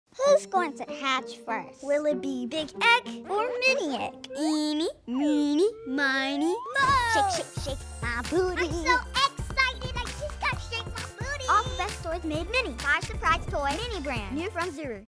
*NEW* Find MINI Rainbocorns in ALL NEW 5 Surprise Toy Mini Brands!
5 Surprise Toy Mini Brands are the newest real brands made MINI. This all new collection features over 70 of the most iconic toys, including your favourite, RAINBOCORNS! Unwrap, peel and reveal your 5 Surprise to find 5 of the most realistic toy minis, how many can you collect? Find them all and build the biggest mini collection.